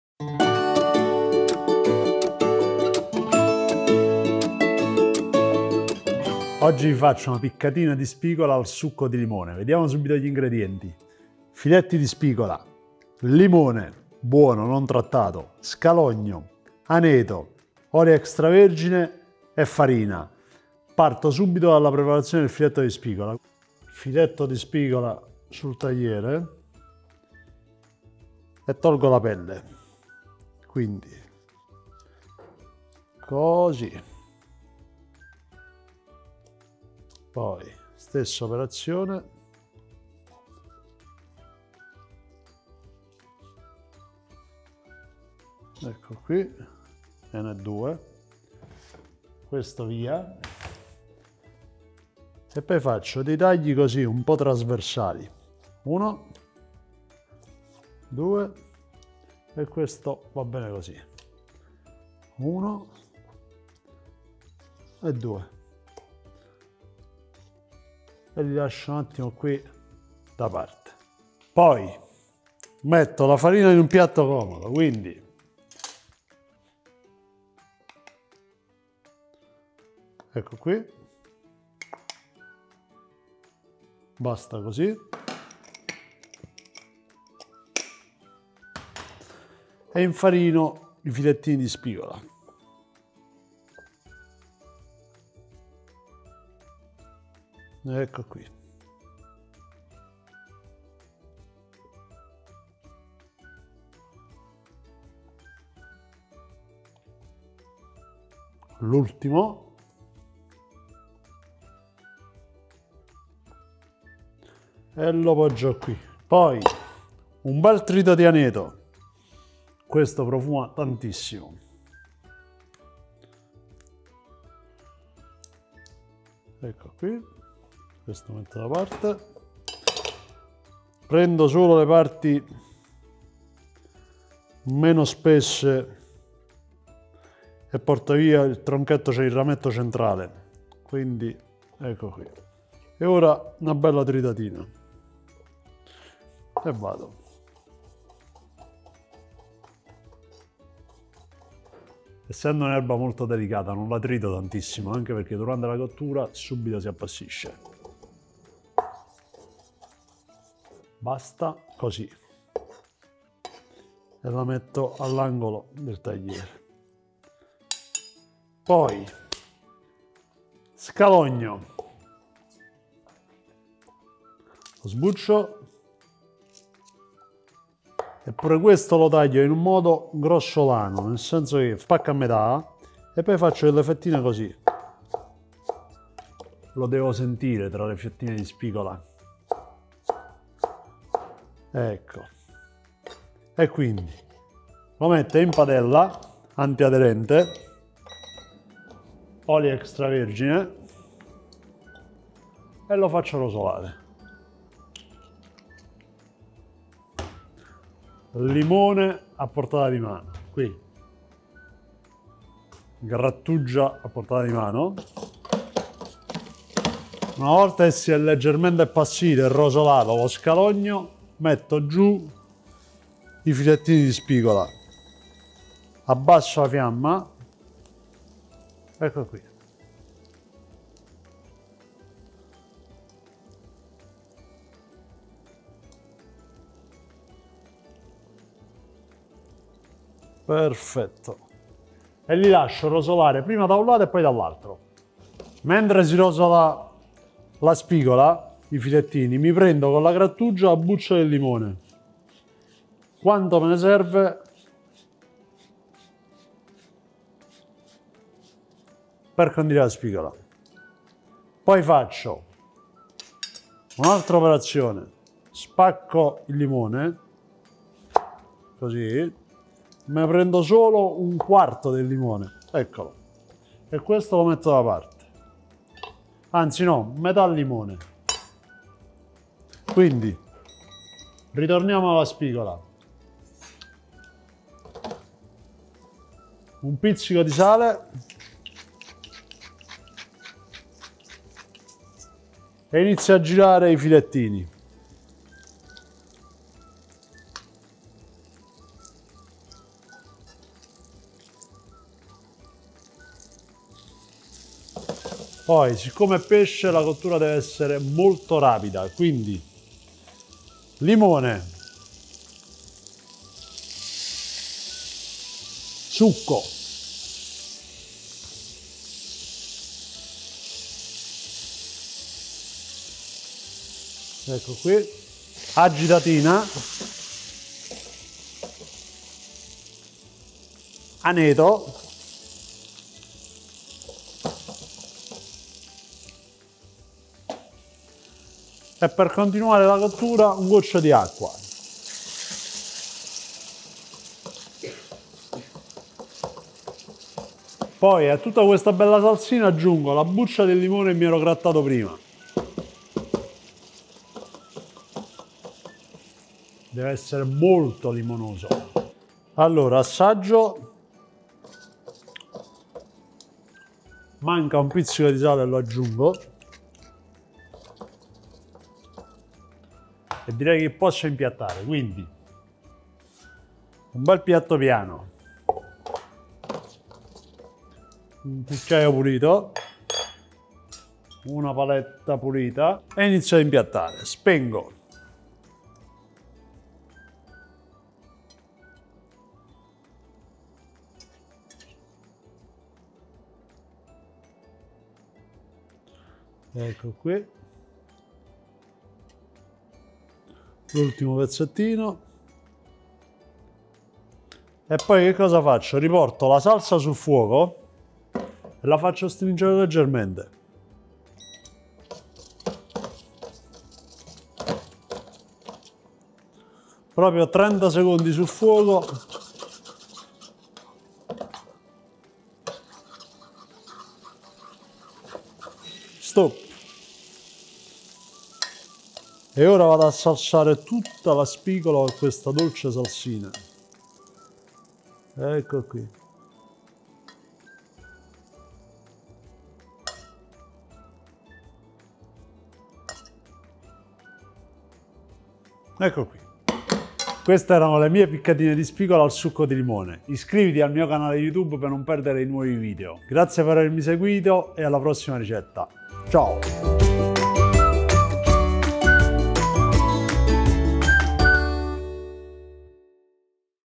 Scaloppine di Spigola 👨‍🍳 Videoricetta Chef Raffaele Lenti
Le piccatine di spigola al succo di limone sono delle scaloppine fatte con il pesce e non con la carne, un piatto ghiotto ,sfizioso e, nello stesso tempo, facilissimo da preparare anche per chi non è tanto esperto in cucina ma voglia comunque cimentarsi nella cottura del pesce.

CLICCA SULLA CAMPANELLA PER ESSERE AVVISATO TUTTE LE VOLTE CHE LO CHEF PUBBLICA UNA NUOVA RICETTA!

💻 SOCIAL!